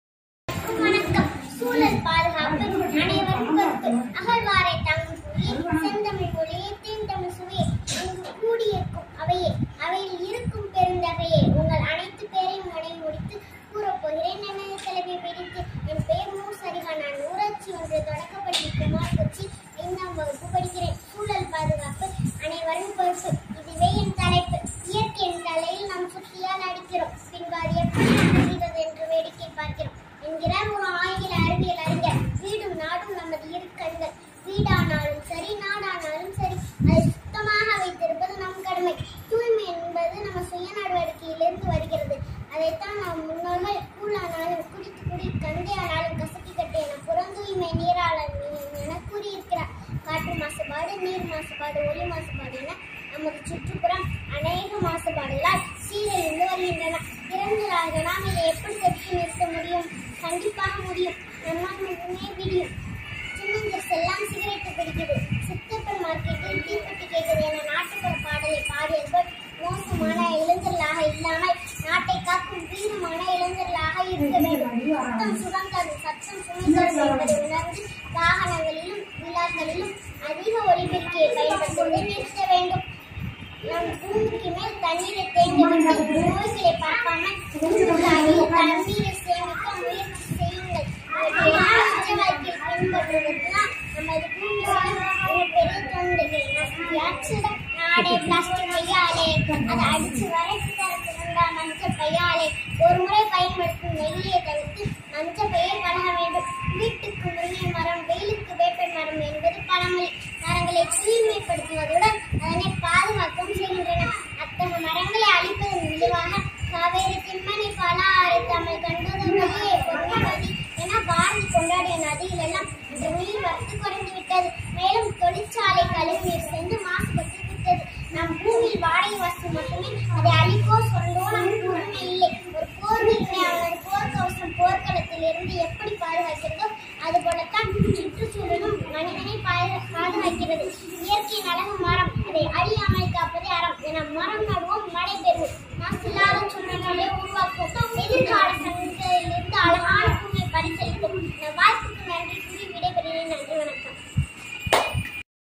சூழல் பாதுகாப்பு என்ற தலைப்பில் ஐந்தாம் வகுப்பு மாணவியின் பேச்சு திறன்youtuber video
சூழல் பாதுகாப்பு என்ற தலைப்பில் ஐந்தாம் வகுப்பு மாணவியின் பேச்சு போட்டி